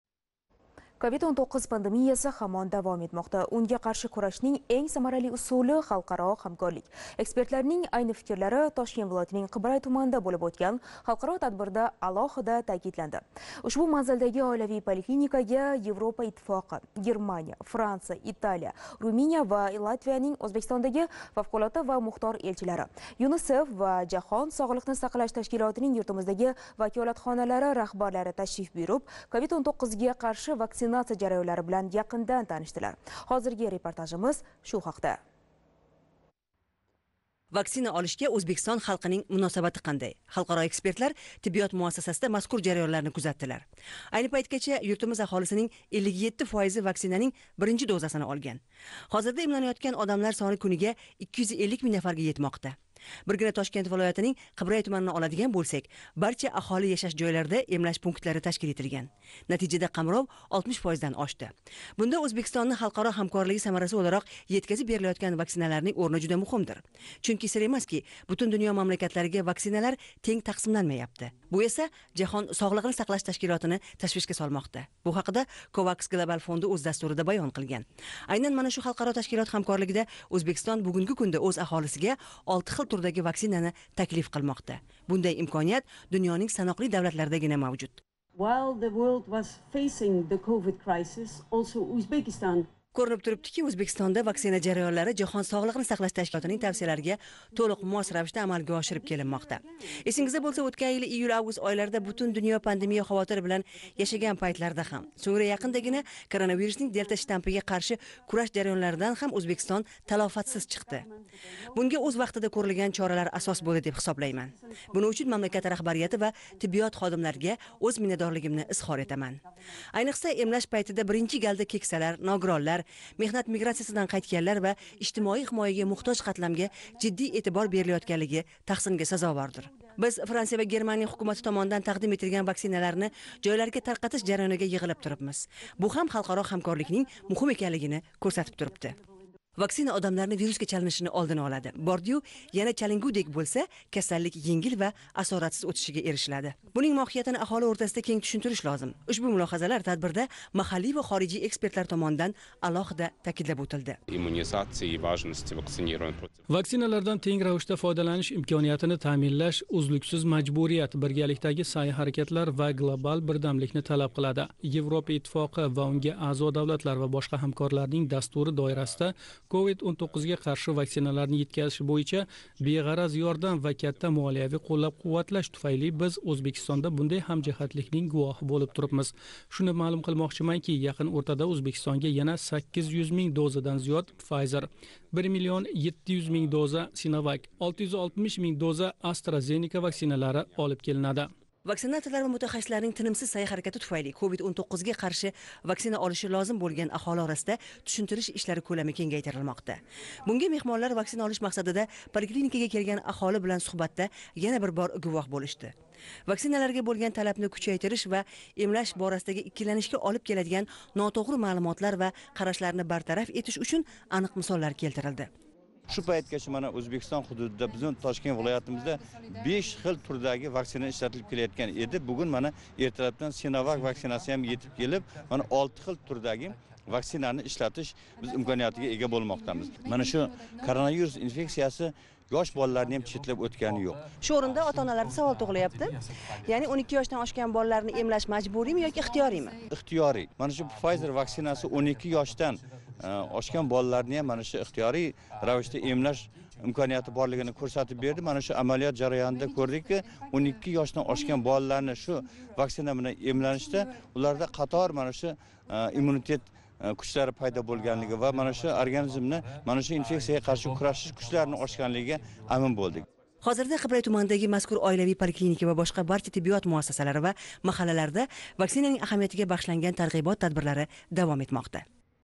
Европа Иттифоқи давлатлари элчиларининг Қибрайдаги поликлиникага ташрифи
Тошкент вилояти ҳокимлиги ахборот хизмати